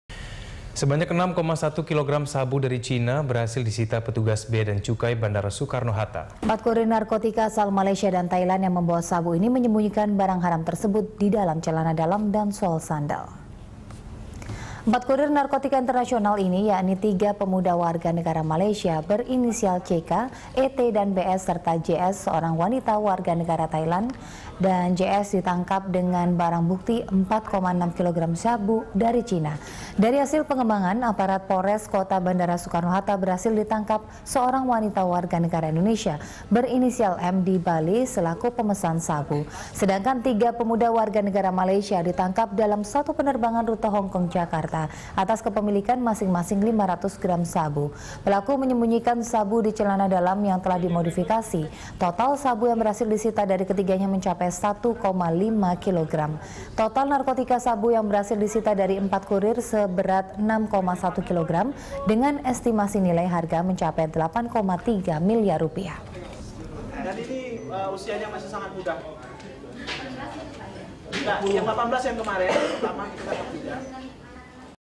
Penyelundupan Sabu Asal China Digagalkan Bea Cukai Soekarno Hatta
Sebanyak 6,1 kilogram sabu dari China berhasil disita Petugas Bea dan Cukai Bandara Soekarno Hatta. Empat kurir narkotika asal Malaysia dan Thailand yang membawa sabu ini menyembunyikan narkoba tersebut  di celana dalam dan sol sandal.

Facebook.com/BeritaSatuTV
Youtube.com/BeritaSatu